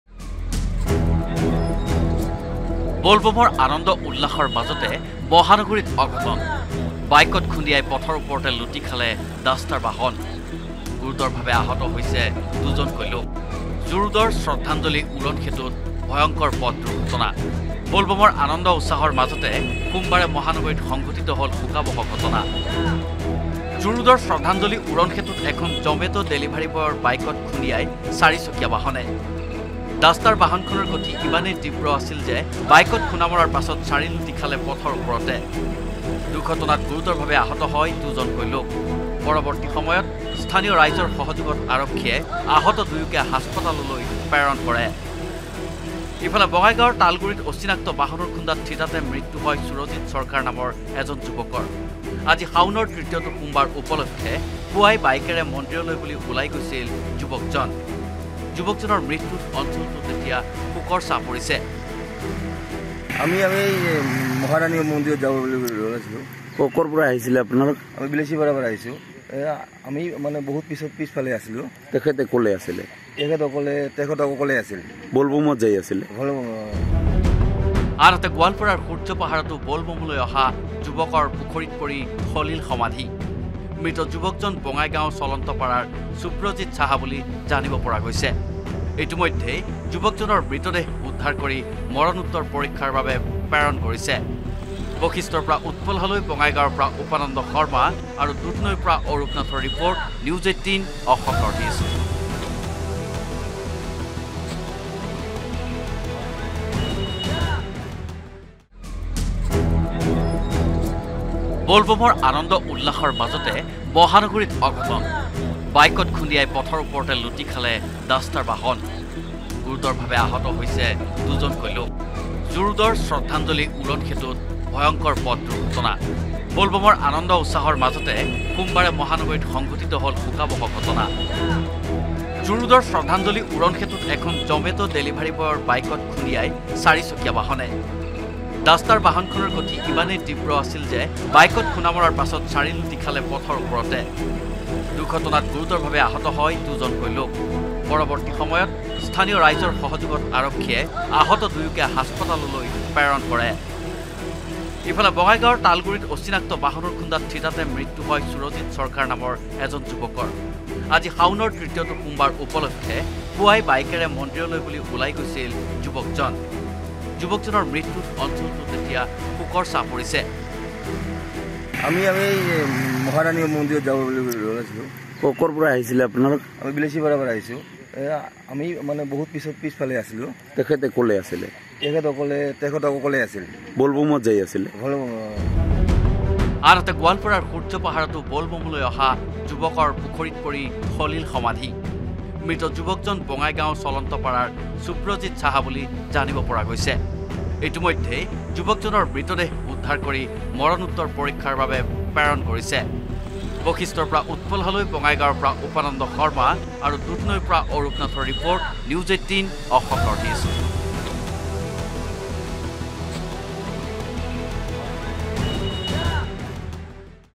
Assam Road Mishap | জু ৰোডৰ শ্ৰদ্ধাঞ্জলি উৰণ সেতুৰ সমীপত দুৰ্ঘটনা | N18V
Assam Road Mishap |  ৰাজ্যৰ প্ৰান্তে প্ৰান্তে সংঘটিত হৈছে ইটোৰ পাছত আনটো দুৰ্ঘটনা। জু ৰোডৰ শ্ৰদ্ধাঞ্জলি উৰণ সেতুৰ সমীপত দুৰ্ঘটনাত পতিত হ’ল বিলাসী বাহন। গোৱালপাৰাৰ সূৰ্য্য পাহাৰতো অঘটন ৷ ব’লব’মত অহা যুৱকৰ পুখুৰীত পৰি সলিল সমাধি৷ ইফালে বোল বোমলৈ  গৈ থকা অৱস্হাত বঙাইগাঁৱত সুৰজিৎ সৰকাৰ খুন্দিয়ালে অচিনাক্ত বাহনে

লগতে চাওঁক দিনটোৰ বহু গুৰুত্বপূৰ্ণ খবৰ |